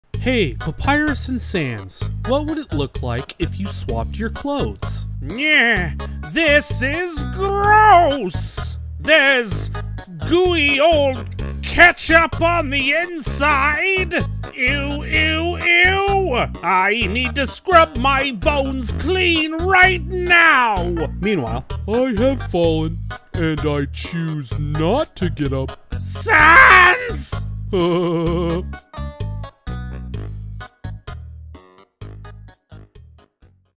Papyrus and Sans Swap Clothes! Undertale Comic Dub!
Papyrus and sans have very different outfits. What if they swapped clothes? Would they still fit?